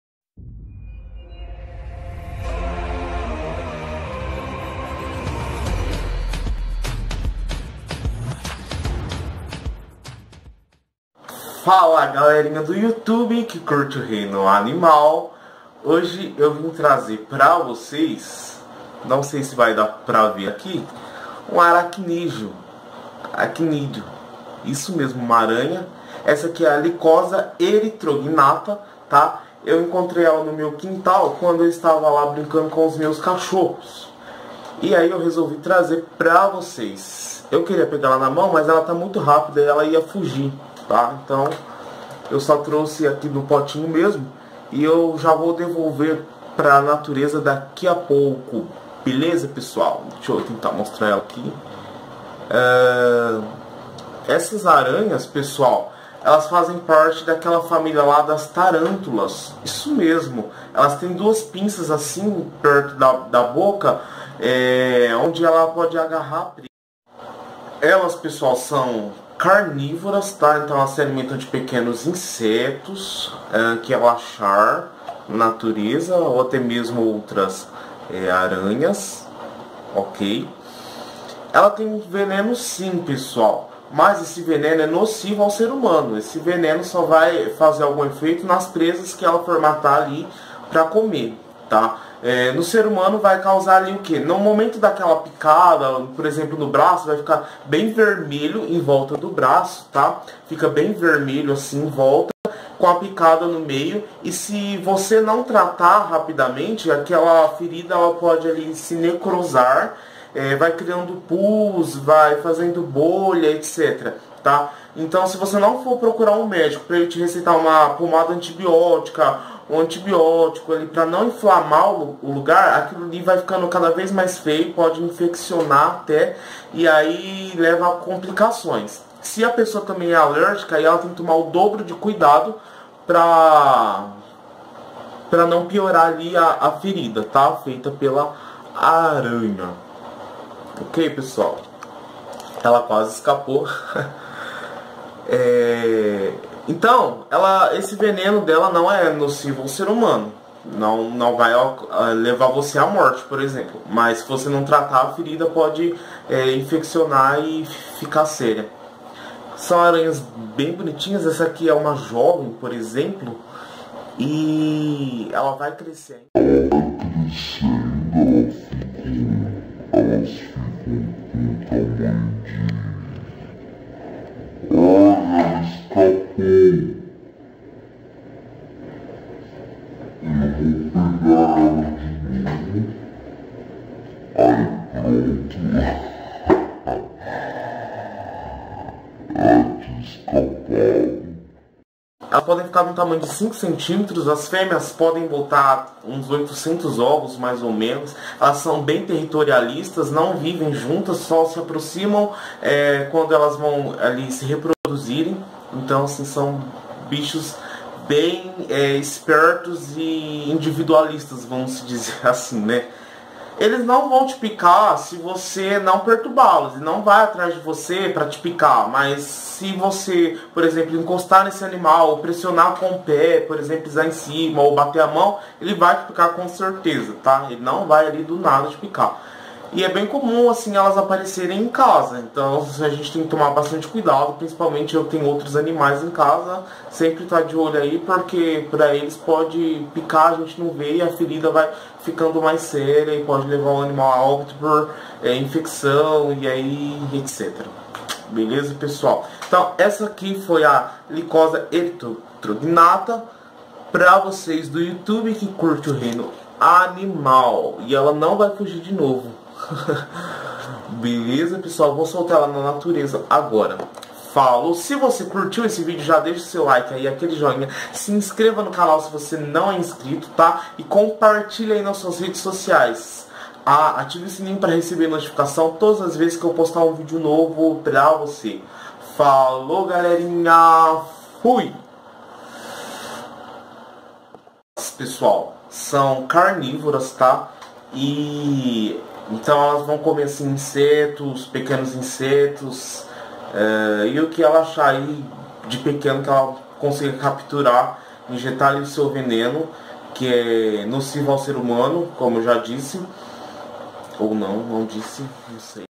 Vlog Animal Radical / Aranha - de - Jardim (Lycosa Erythrognatha)
Lycosa Erythrognatha é uma aranha popular em residências, conhecida também como aranha - de - grama ou aranha - lobo. Sua toxina não é nociva ao ser humano, ou seja, não o leva a morte.